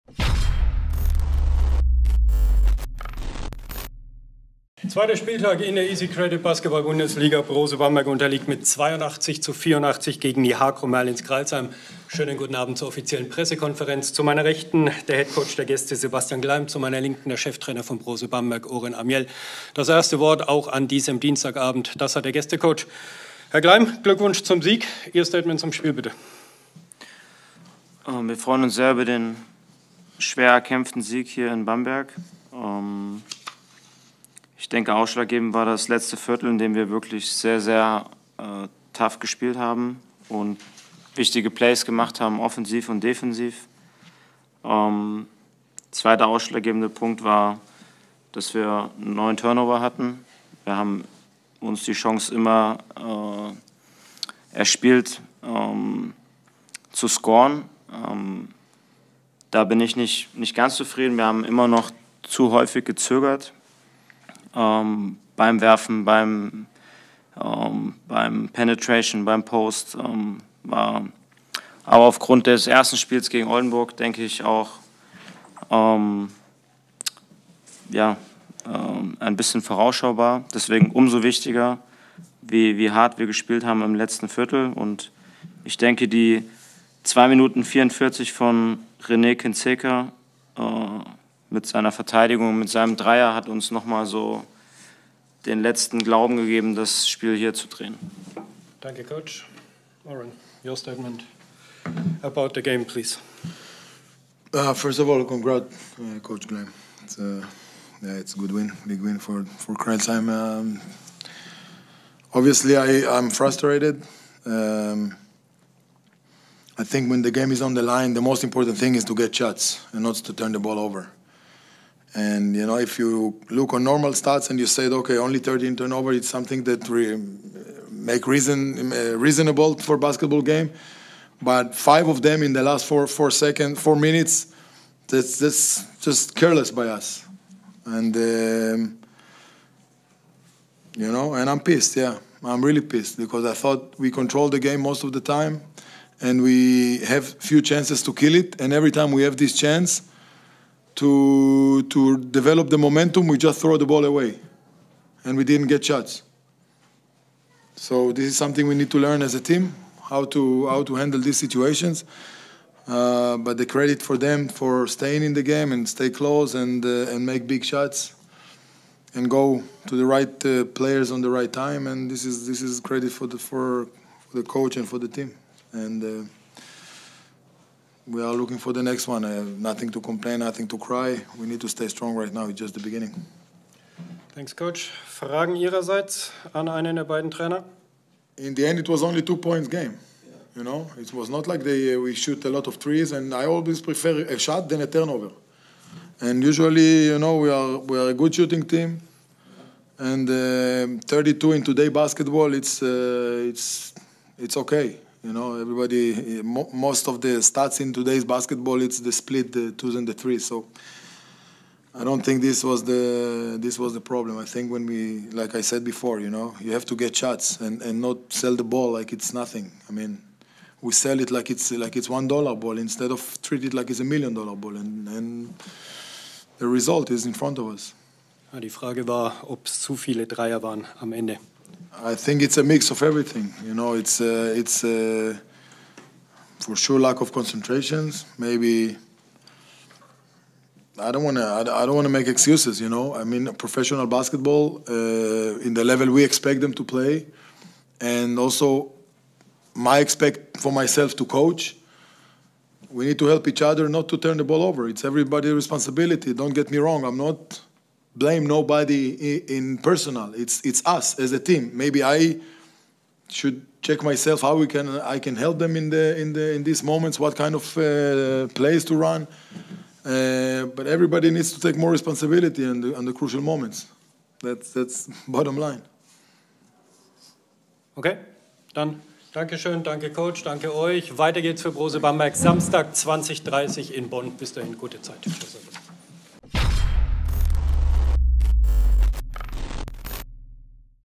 Pressekonferenz Brose Bamberg vs. HAKRO Merlins Crailsheim
Brose Bamberg unterliegt am 2. BBL-Spieltag Crailsheim mit 82:84. Sebastian Gleim und Oren Amiel mit den Statements nach dem Spiel.

Alle Infos rund um Brose Bamberg gibt's unter brosebamberg.de und auf den Social Media Kanälen: